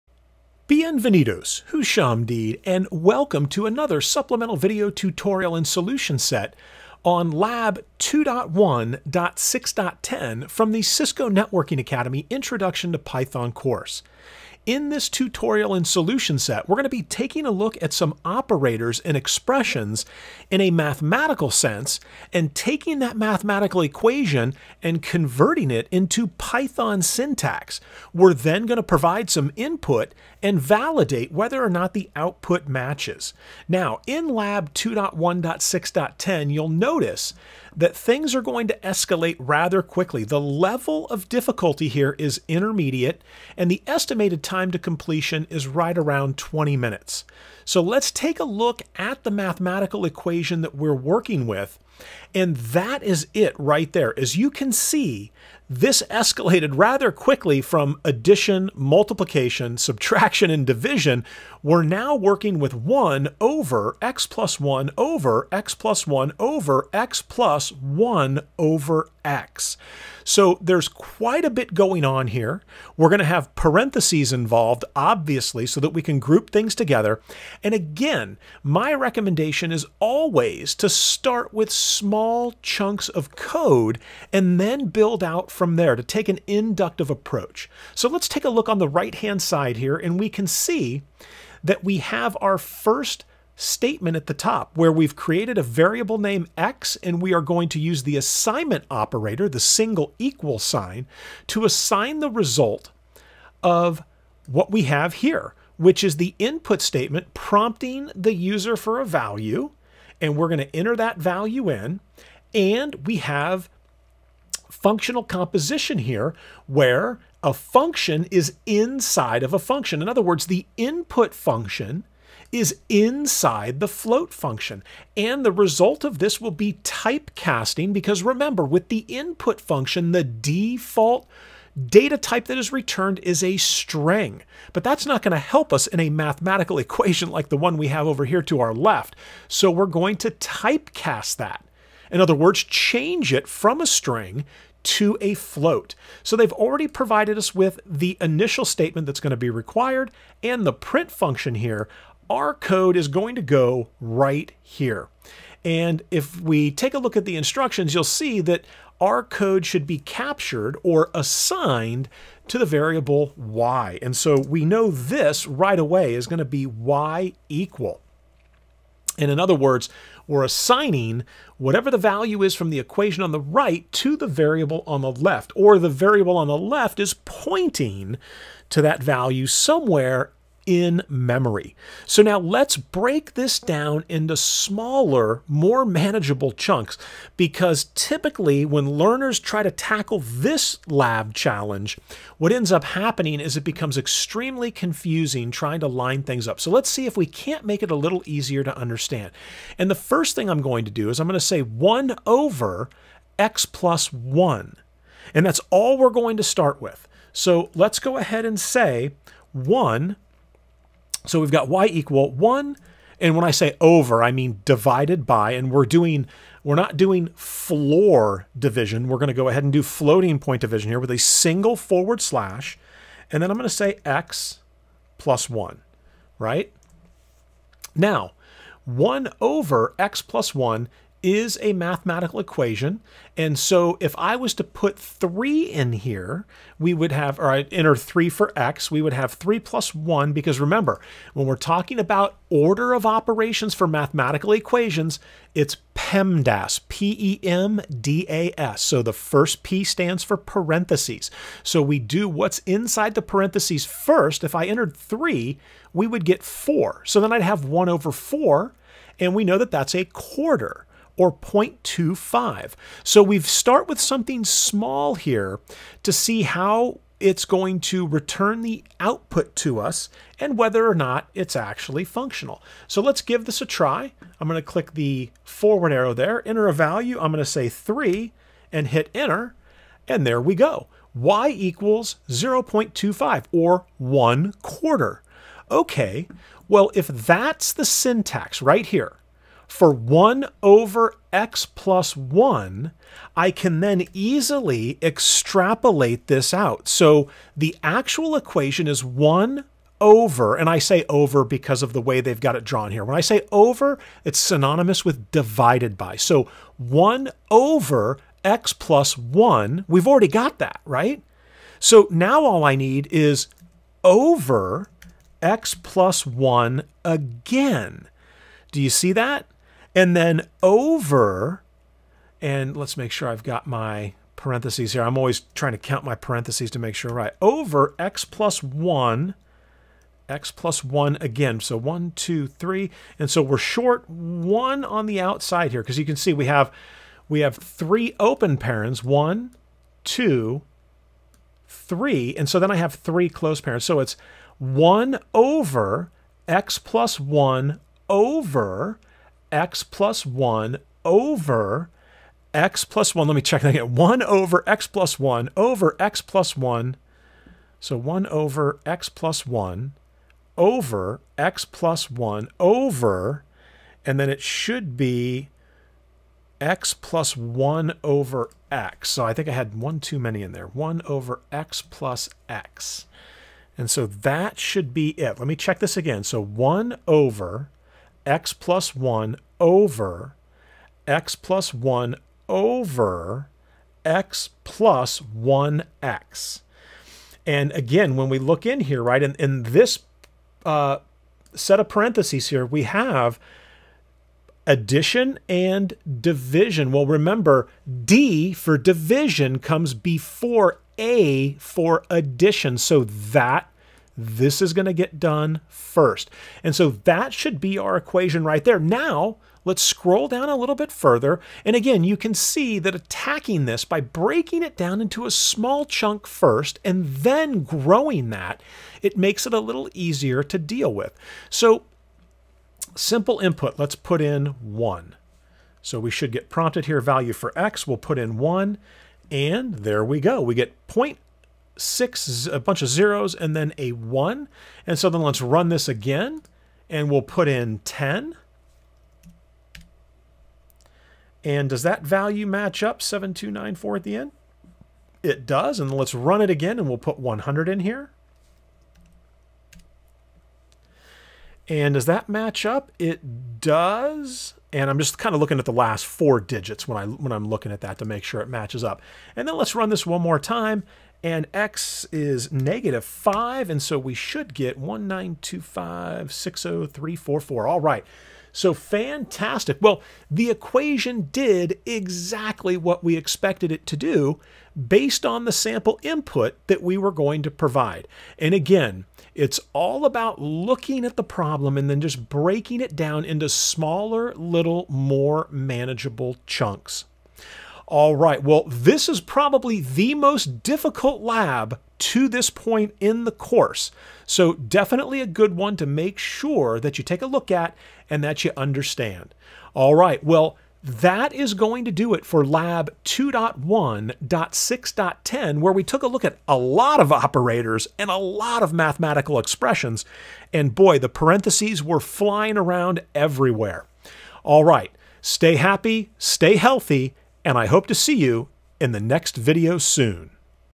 Cisco NetAcad Introduction to Python Course - Supplemental Lab Tutorial & Solution Set: Lab 2.1.6.10
In this series of Python tutorials we will be covering all of the labs from the Cisco Networking Academy 'Introduction to Python Course'! I am currently teaching this course to a group of NetAcad instructors and wanted to add some value to their studies as well as yours! Hope you enjoy these lab tutorials and here is the link I references in the video for the tutorial I posted on how to sign up for this exact same course FOR FREE at the Python Institute! Here is the link: